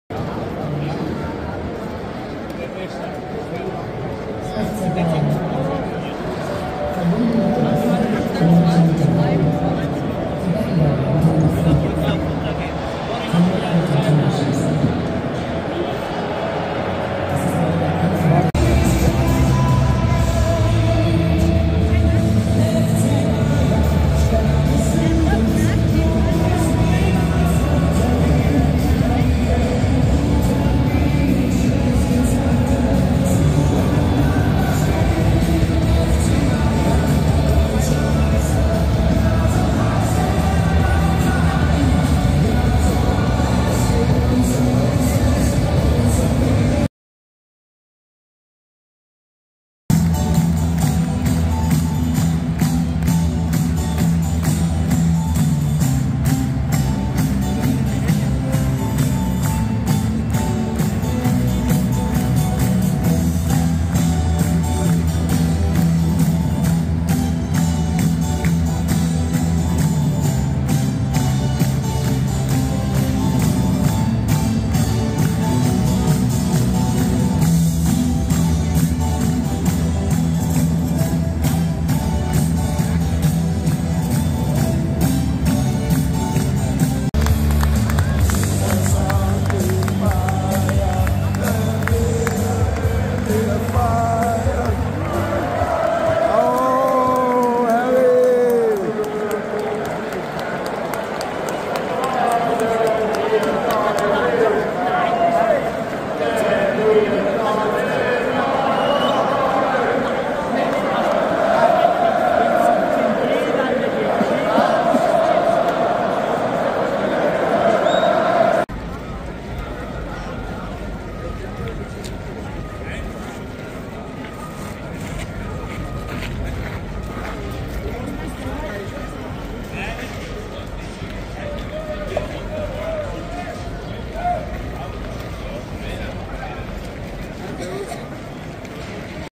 Allianz Arena Bayern München gegen RB Leipzig | Die Schönbuch Gams war live dabei
Ich wurde gestern eingeladen zum Bayern München Spiel gegen RB Leipzig in die Allianz Arena und es war ein mega Erlebniss das mal Live zu sehen und es gab ein Sieg gegen Leipzig